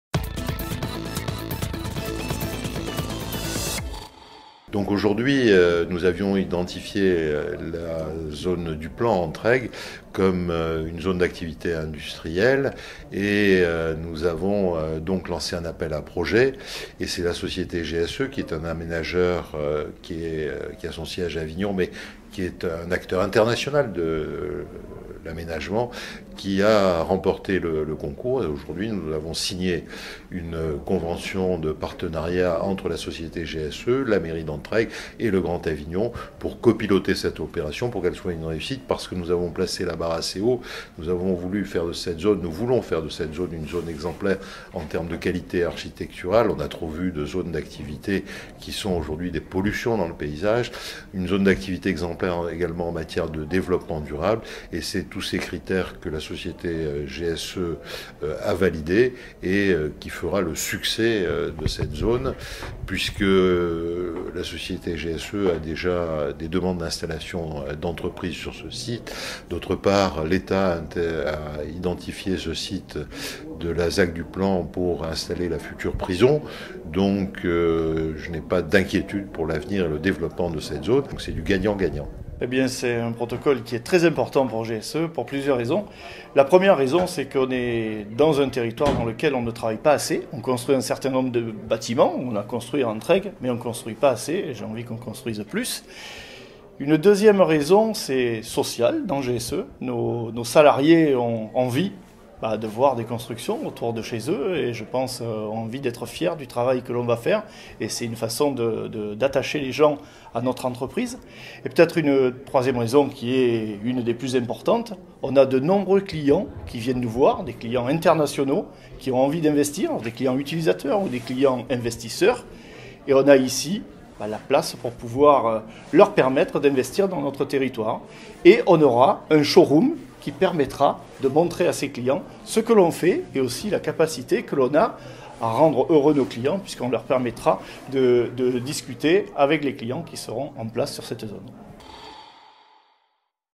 Grand Avignon : L’extension de la ZAC du Plan à Entraigues officiellement lancée
Le Grand Avignon, la commune d’Entraigues-sur-la-Sorgue et la société GSE viennent de signer un protocole d’accord portant sur l’extension du parc industriel du Plan. Une gouvernance tripartite pour l’aménagement de cette future zone à vocation économique qui s’étendra sur 127 hectares supplémentaires et devrait être porteuse de milliers d’emplois.

Reportage : Nicolas François